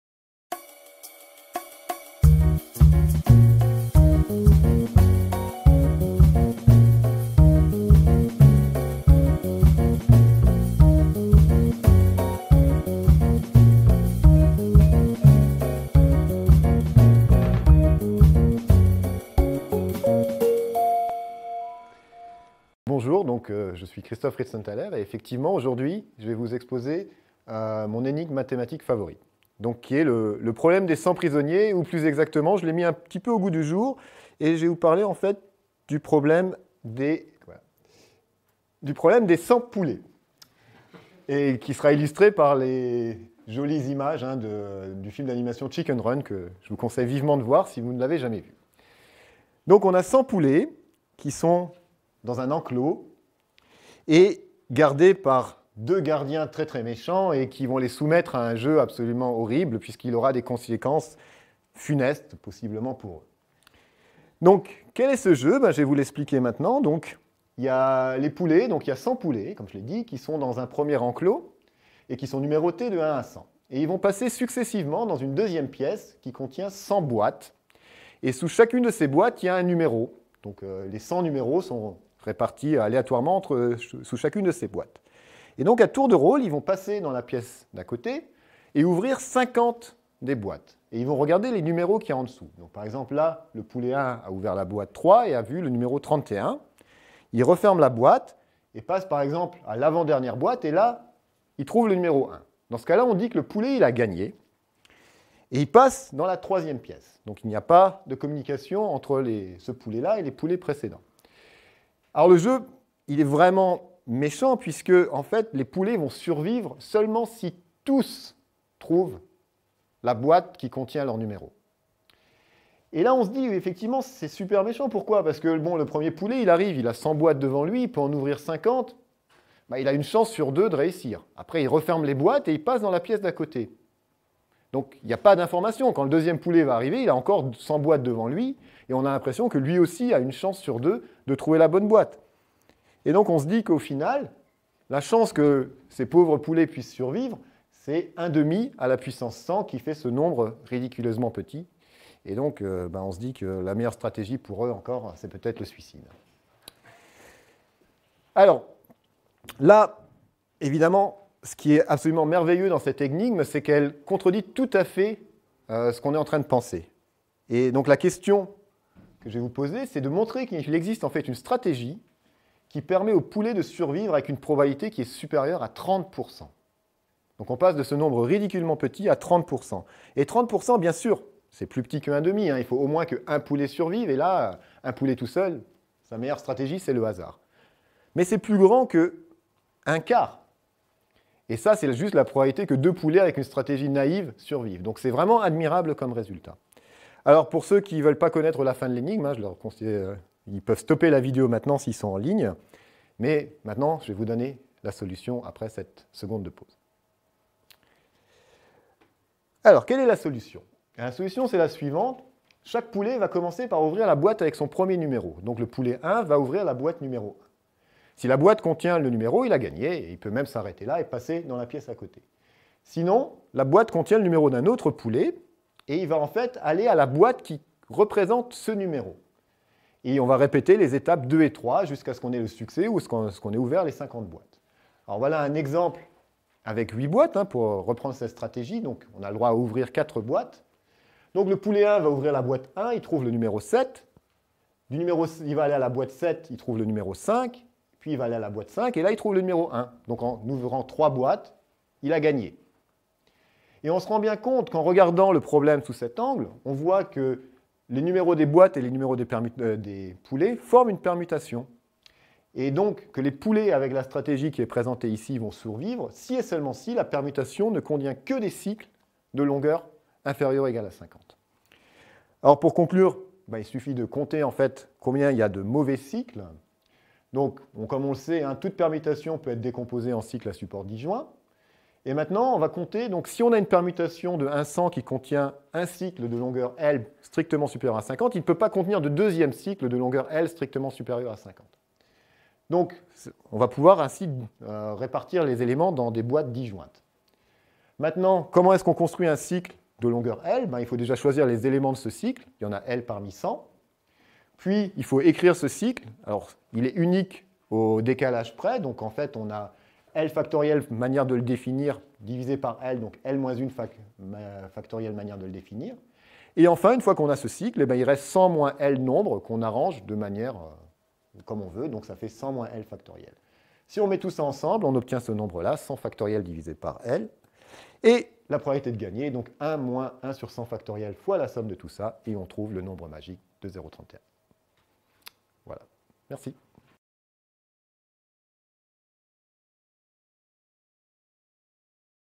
Christophe Ritzenthaler - Les cent prisonniers
Nous exposerons cette jolie énigme mathématique ainsi que sa solution basée sur de la combinatoire des permutations